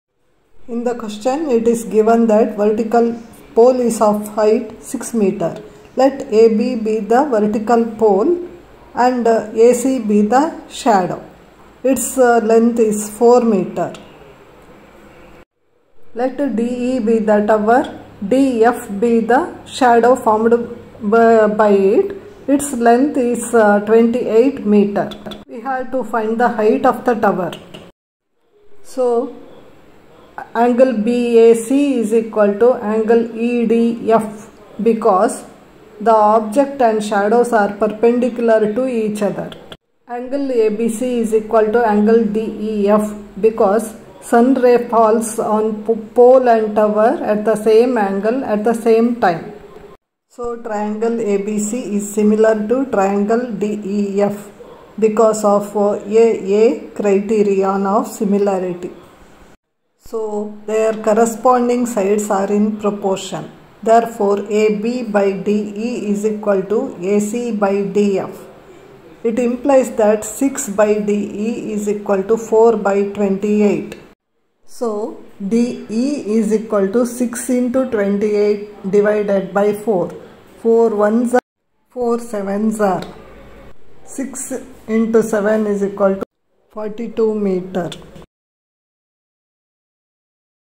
A vertical pole of height 6 m casts a shadow 4 m long on the ground, and at the sslcexam2023
A vertical  pole  of  height  6 m casts a shadow 4 m long  on the ground, and  at the same time a tower on the same ground casts a shadow 28 m long.  Find the height of the tower.
triangles
aa similarity criterion

2019 supplementary exam question
2 mark